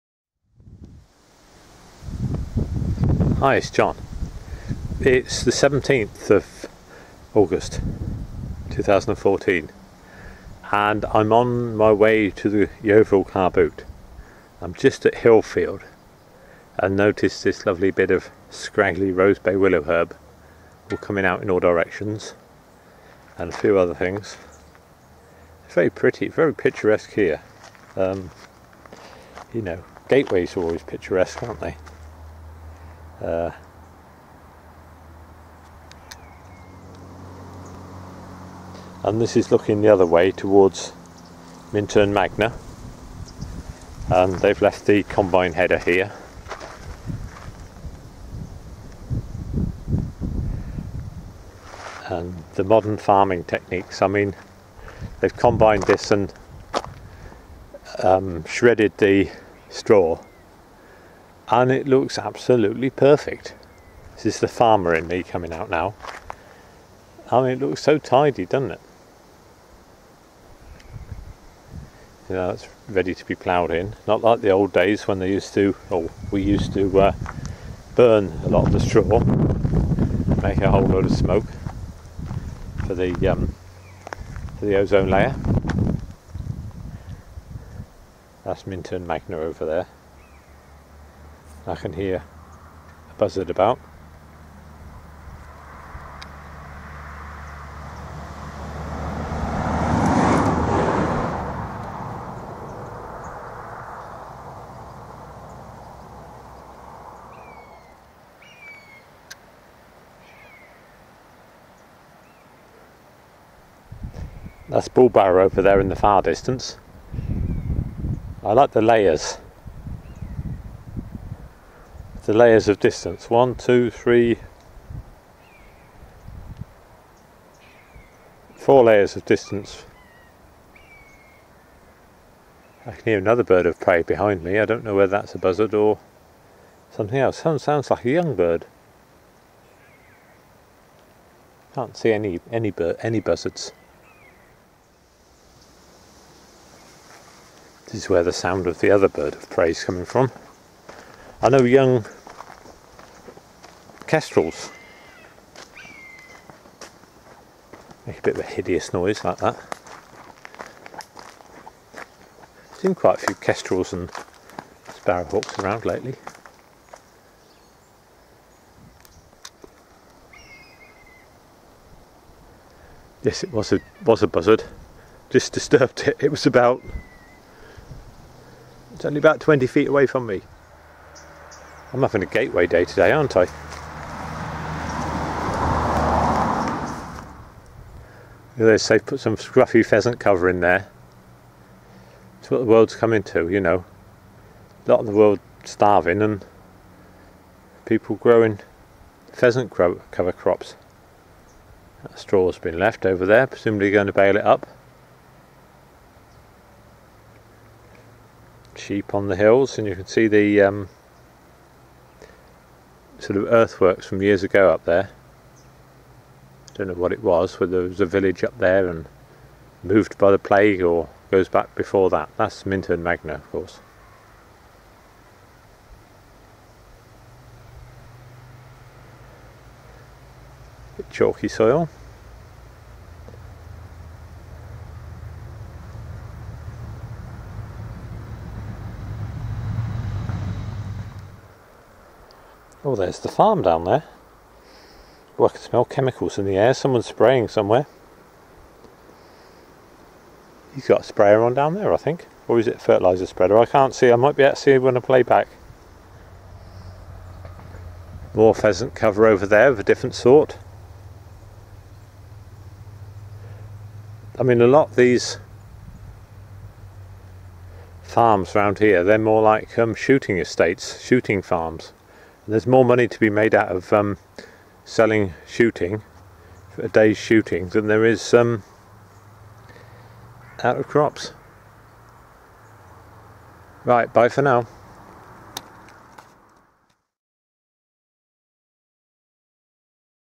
Views from Hillfield/Mintern Magna Ridge, England, UK, on the way to Yeovil
There is a bite in the air, the wind is coming from the North, most of the corn is now harvested and the seasons move on.  I am beginning to get to know the area well, partly through doing all these videos. I'm still getting used to the Panasonic Camera, it is easier to use than the vivitar but it has more unexplored features.
Filmed using Panasonic:HC-V100